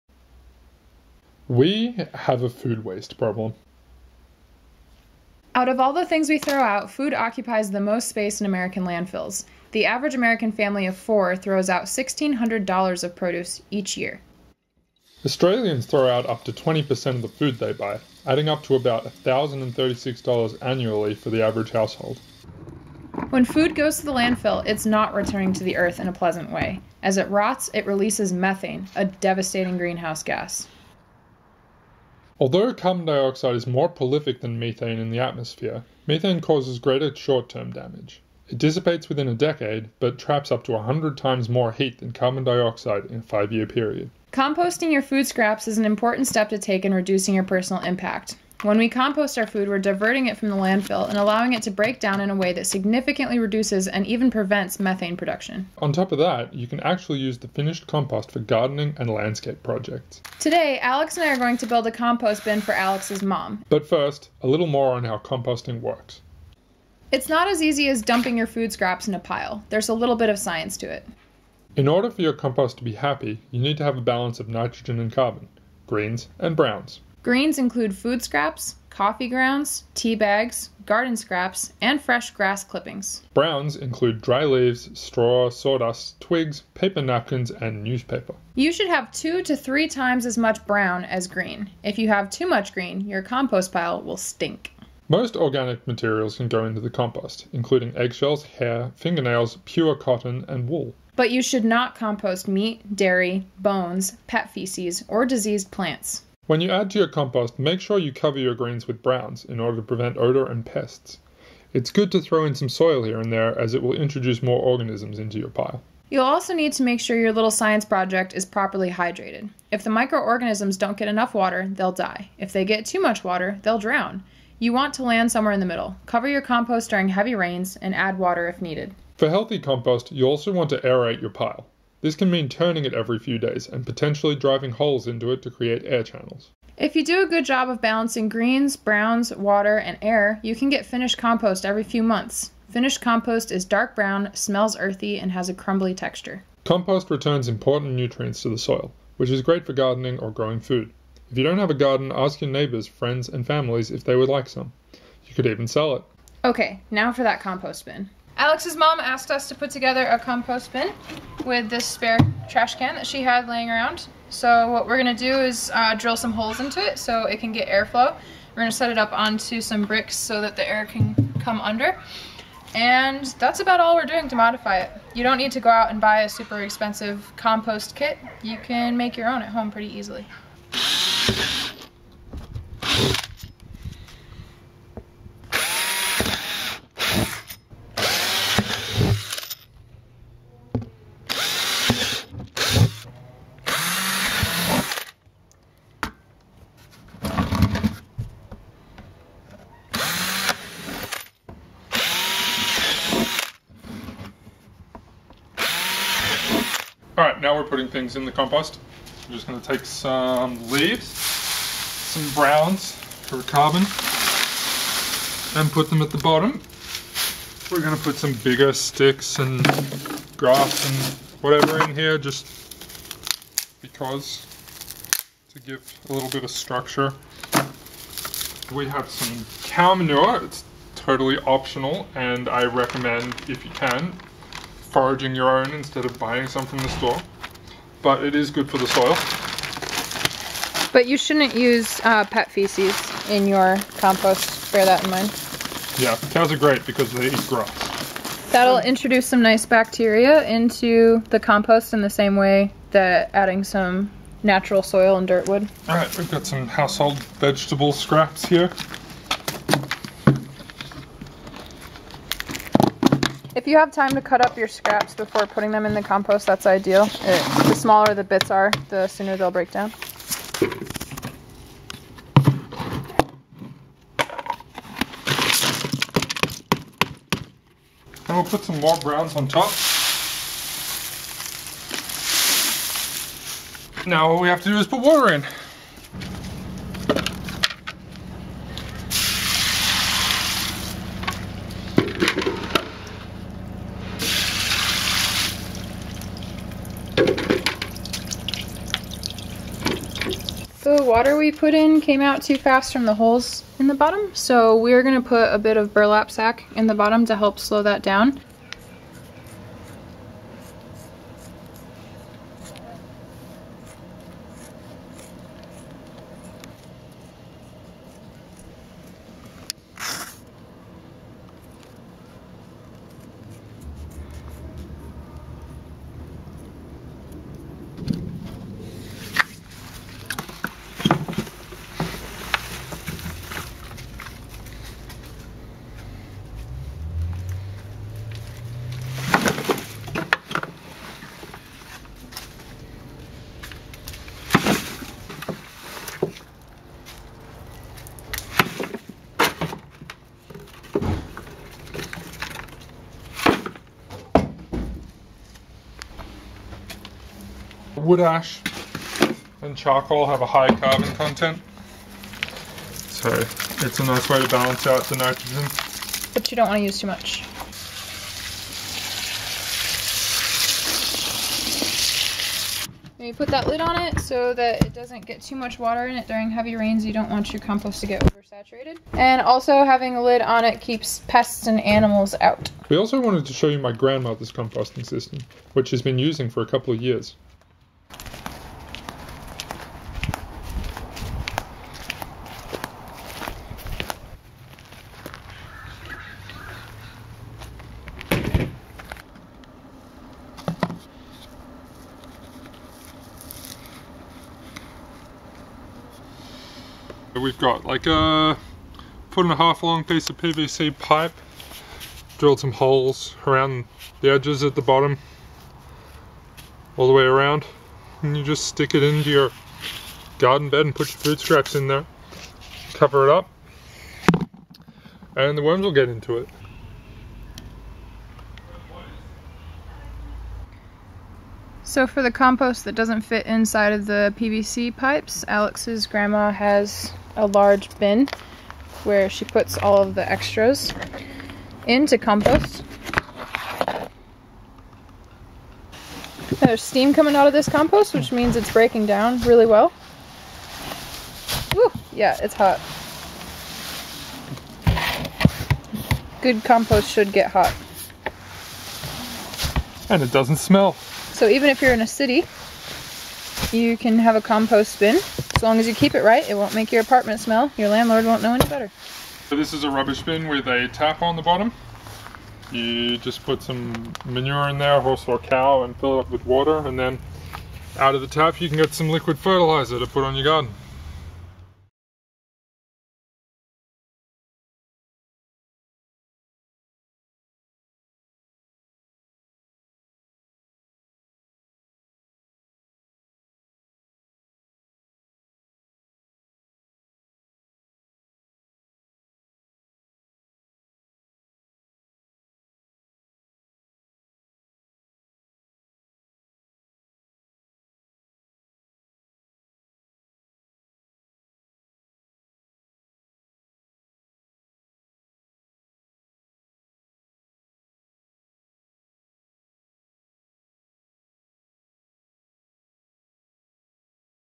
How to start a compost bin
In this video we show you how to get a proper compost bin started so you can reduce your food waste and make use of your scraps. When we send food to landfills, it produces methane — a greenhouse gas — as it breaks down with other trash. But when we compost food, we avoid producing methane and give a helping hand to the planet.

and Twitter: @EarthlingDream